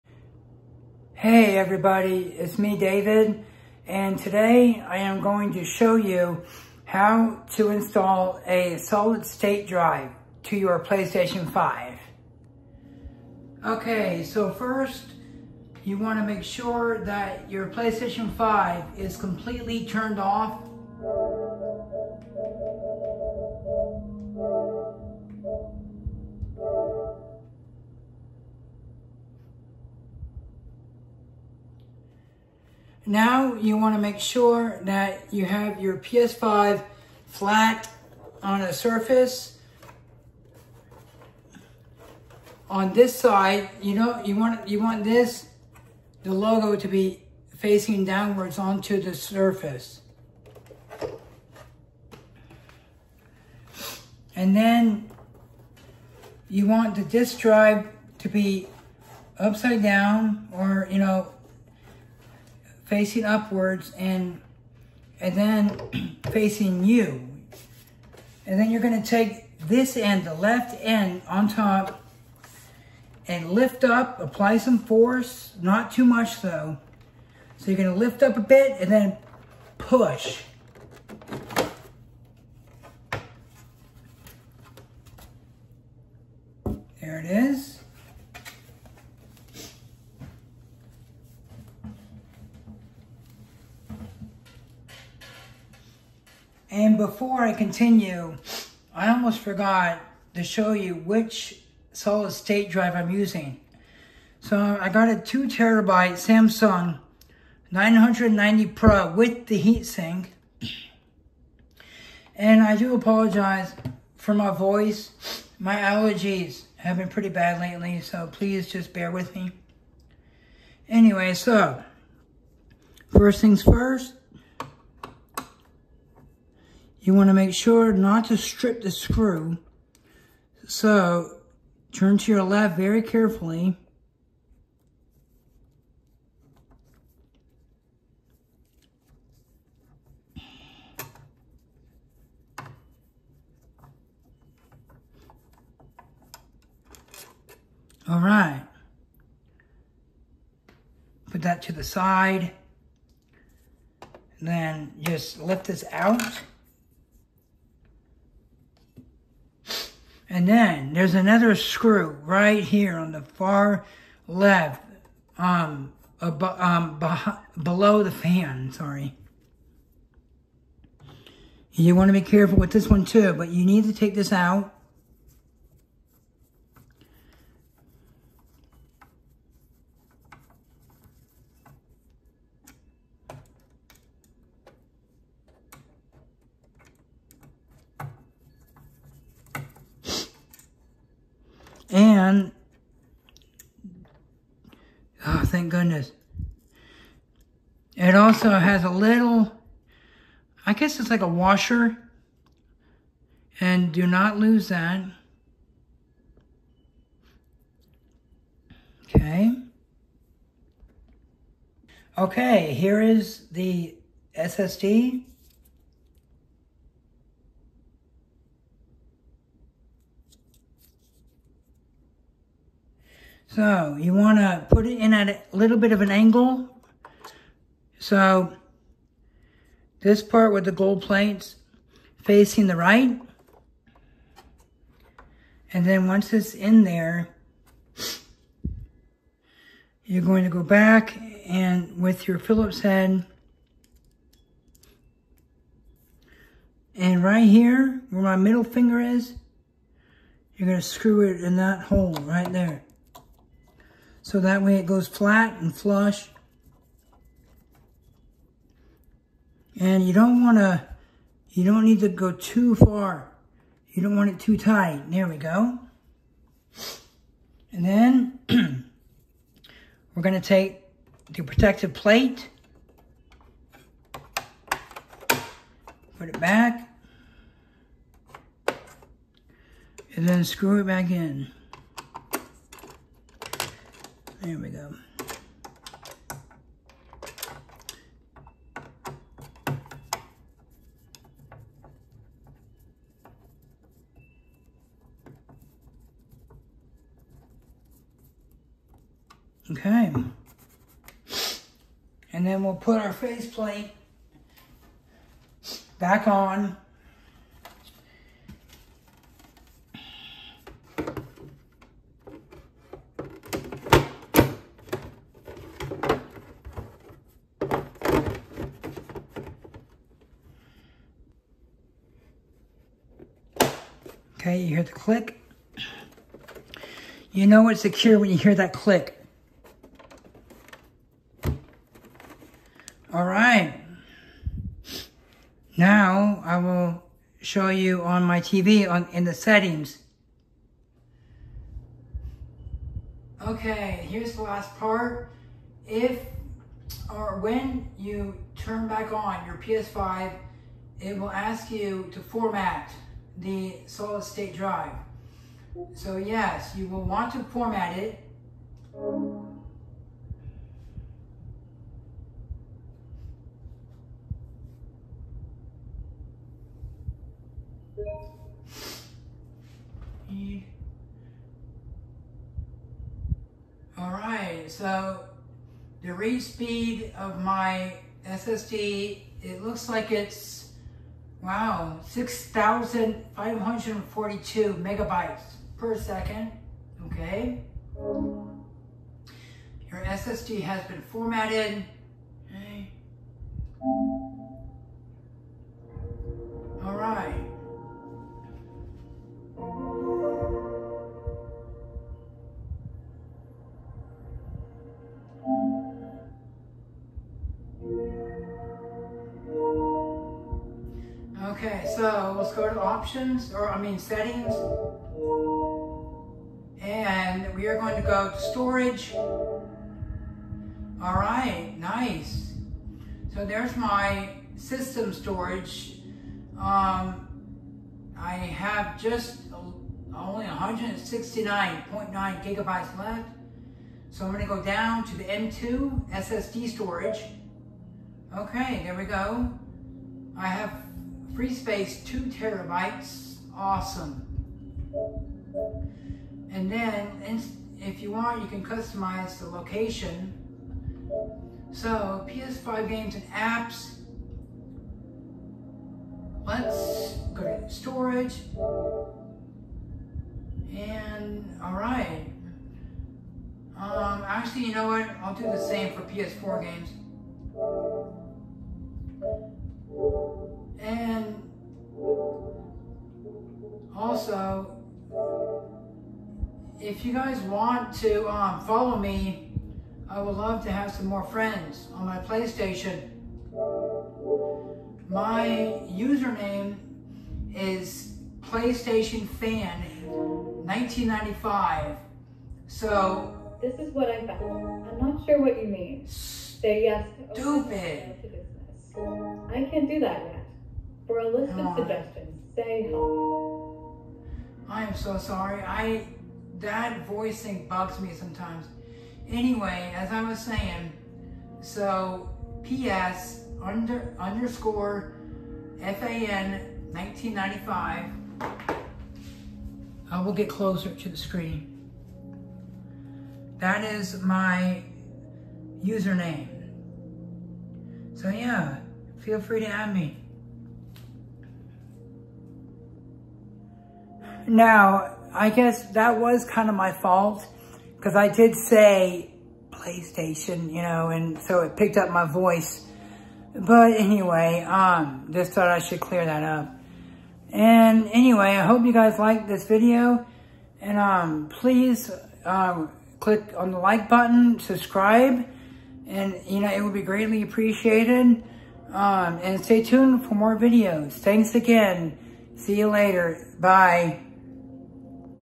How to install and setup a Samsung 990 pro SDD with Heatsink on a PlayStation 5
I do an in depth easy walkthrough on how to install and setup a Samsung 990 pro with heatsink SSD onto a PlayStation 5